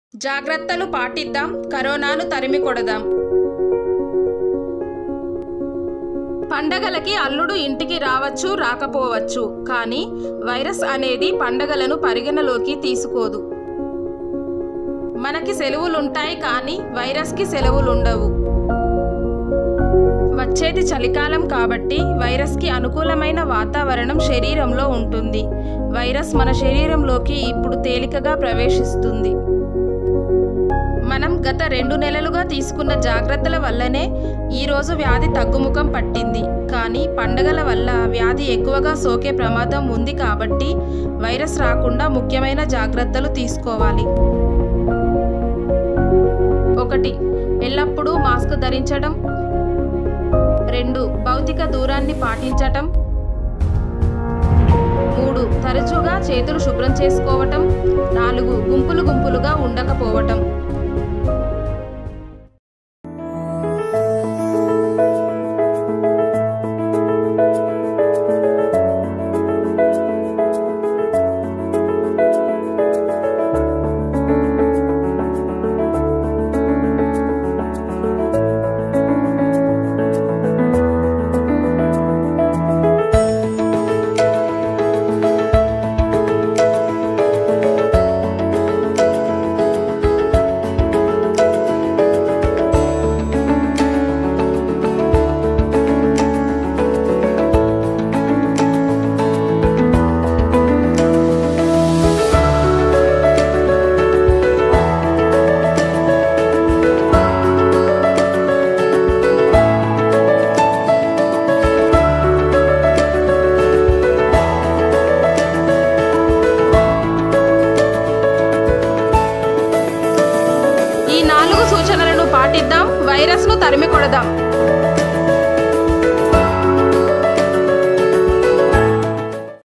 చలికాలంలో కరోనా వైరస్ తీవ్రత అధికంగా ఉంటుందా? Alert.. Virus attack On Winter Season
ఈ చలికాలంలో కరోనా వైరస్ కి మన శరీరం అనుకూలంగా ఉంటుంది. ఇప్పుడున్న పరిస్థితుల్లో వైరస్ తీవ్రత అధికంగా ఉంటుంది కావున మనం ఎప్పటికప్పుడు వైరస్ నియంత్రణ చర్యలు పాటించాలి.

                 ◆    శ్రీ బి. నరేష్
◆జిల్లా సంక్షేమ అధికారి-జగిత్యాల జిల్లా

             ◆    శ్రీ కె.లక్ష్మీనారాయణ
◆జిల్లా గ్రామీణాభివృద్ధి అధికారి-జగిత్యాల జిల్ల

                     ◆   శ్రీ పి.శ్రీధర్
◆జిల్లా వైద్య ఆరోగ్య శాఖ అధికారి-జగిత్యాల జిల్లా

                 ★ శ్రీ జి.రవి IAS ★
◆జిల్లా కలెక్టర్&మేజిస్ట్రేట్-జగిత్యాల జిల్లా.